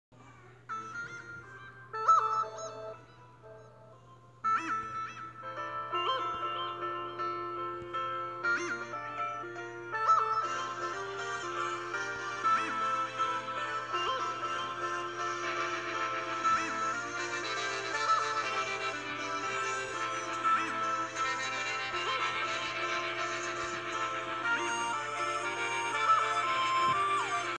My first deviantart account
I had made my first deviantart account in deviantart

The audio your hearing is not mine,this audio came from Lily morgo's yene-times